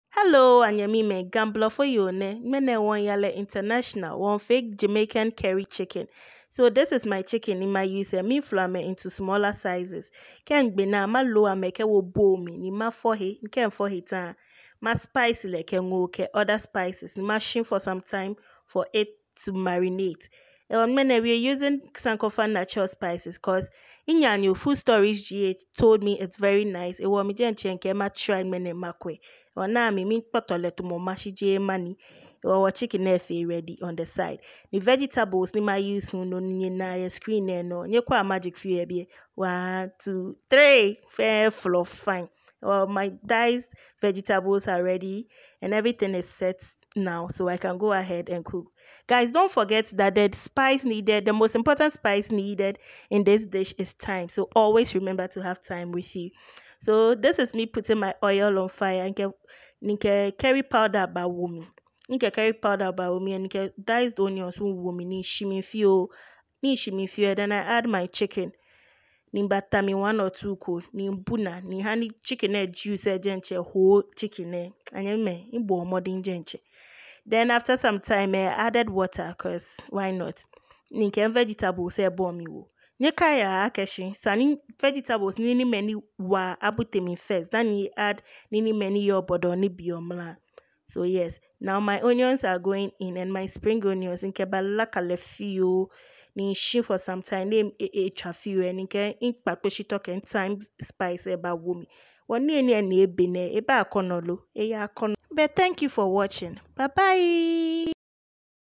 How to make JAMAICAN CURRY Chicken RECIPE
GA Voiceover | Jamaican Curry Chicken Recipe
Let's Make Jamaican curry chicken and yes we are speaking Ga.
Ga is a local dialect in Ghana West Africa, spoken mainly by the people from Accra